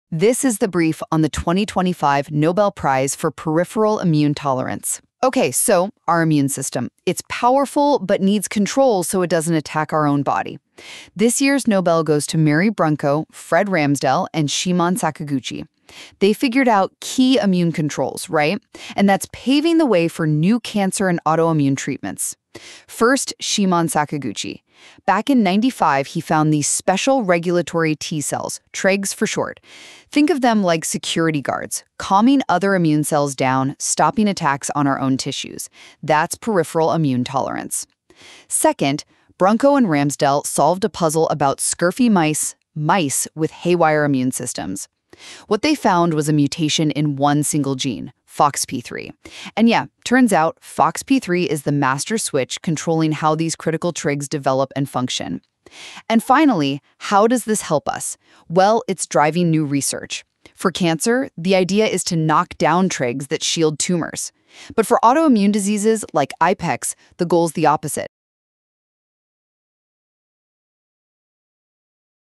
2025 Nobel Prize in Medicine 🧬 The Gene That Controls Your Immune System
2025 Nobel Prize in Medicine 🧬 The Gene That Controls Your Immune System

🧾 Description (Shorts):

The 2025 Nobel Prize in Medicine just revealed how your body stops itself from self-destructing.

Mary Brunkow, Fred Ramsdell, and Shimon Sakaguchi discovered regulatory T cells (Tregs) — the peacekeepers of your immune system — and the FOXP3 gene, the master switch that keeps it all in balance.

From fighting cancer to curing autoimmune disease — this tiny cell could change everything.

Nobel, Medicine, Immune, Tregs, Cancer, Discovery